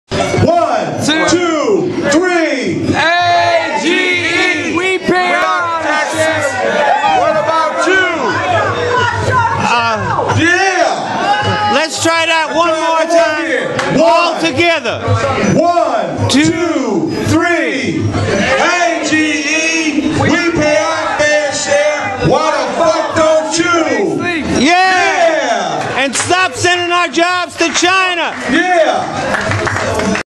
Honest John - 108 People Make a Fair Share Video at the Comedy Show
Corporations should pay their fair share